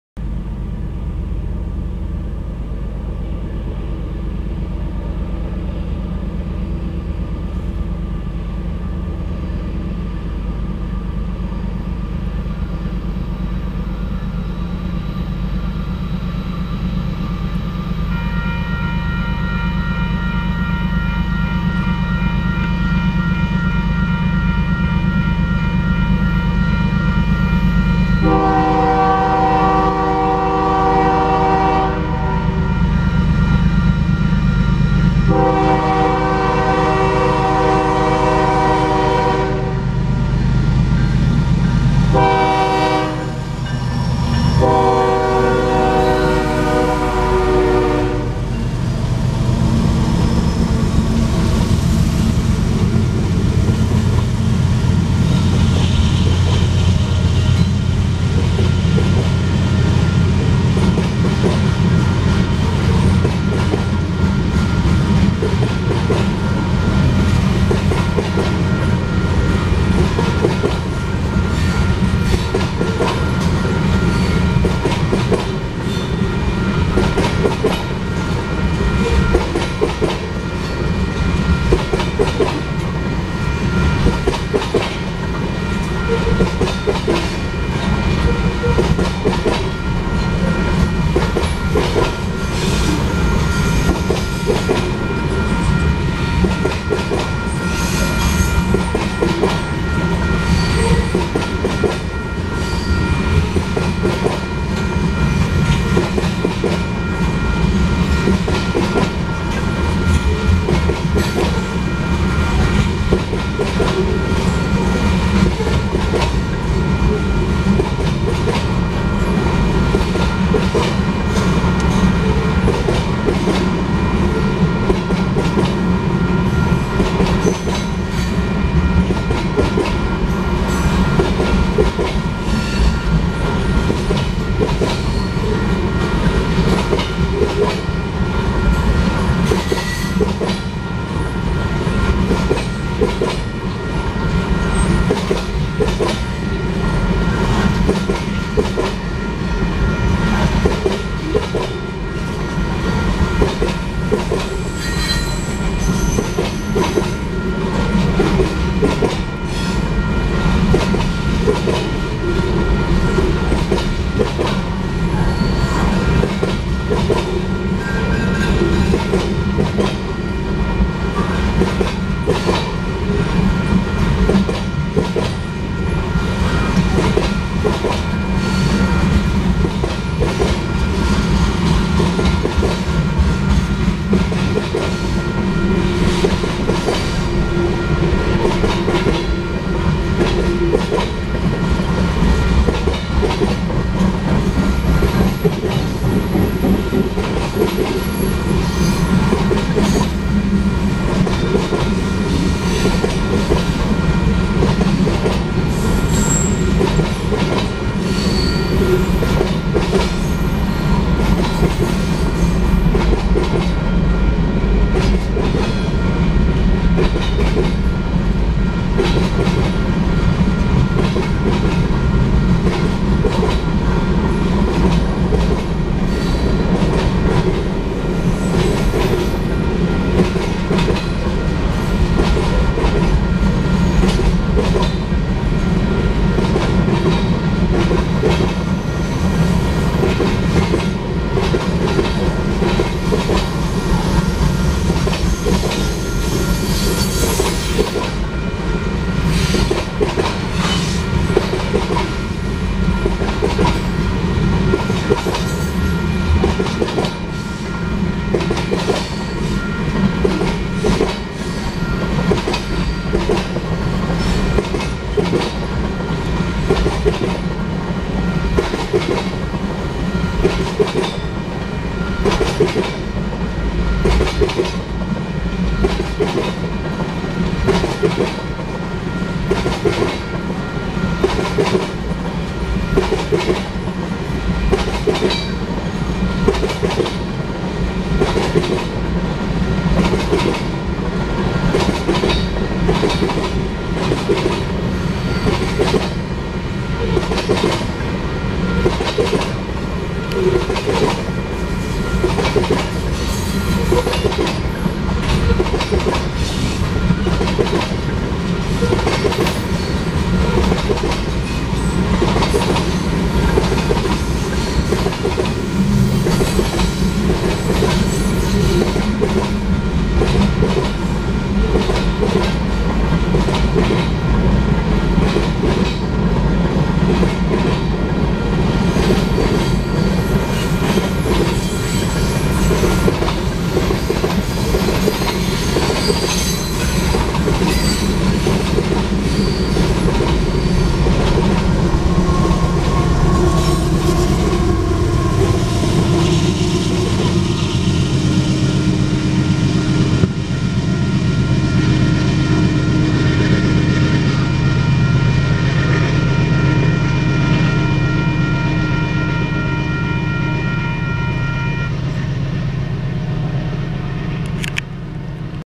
UP 5347 South MITDM (3-27-2020)
South Manifest/Key Train Itasca, WI-Des Moines, IA/Main Track/DOT#876382T-M.P. 296.35
UP Albert Lea Sub/227th St. East Grade Crossing-No. Kasper Siding-Walcott Township, MN/9:03am